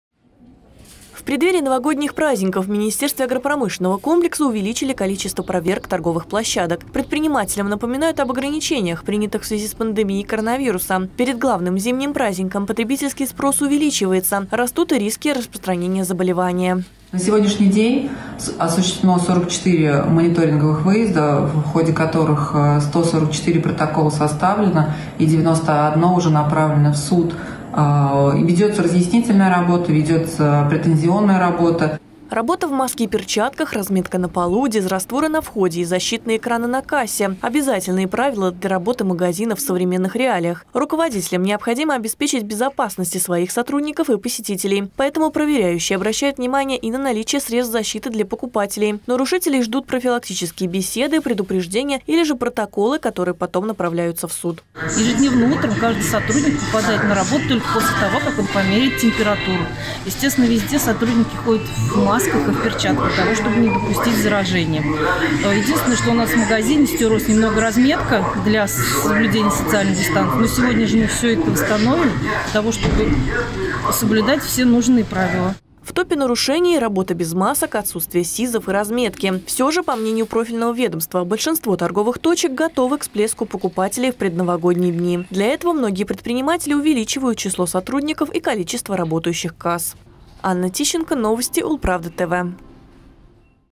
К Новому году готовы? В ульяновских магазинах проверяют ковид-безопасность
В преддверии праздников в министерстве агропромышленного комплекса увеличили количество проверок торговых площадок. Предпринимателям напоминают об ограничениях, принятых в связи с пандемией коронавируса.